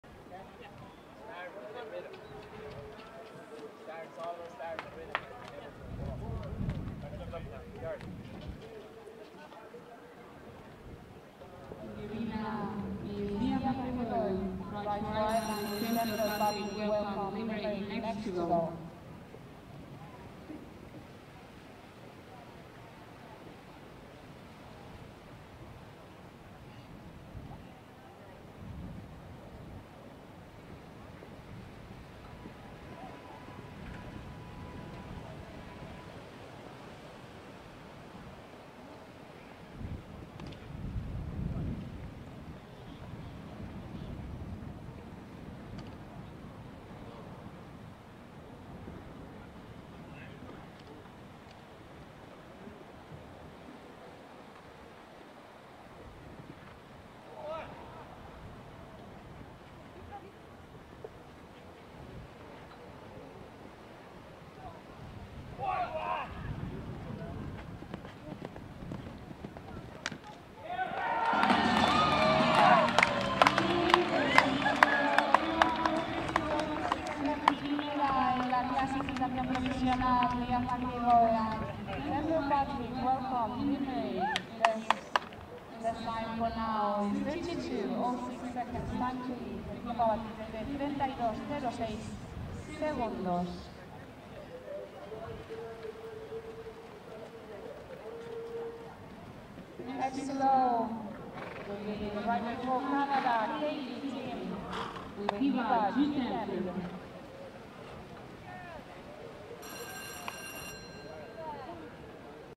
CSIO3* Montenmedio: Vídeo del triunfo de Niamh McEvoy en el Gran Premio
Recorrido de desempate de Niamh McEvoy y “Templepatrick Welcome Limmerick”, ganadores del Gran Premio del CSIO3* que bajó el telón a la segunda edición del Andalucía October Tour.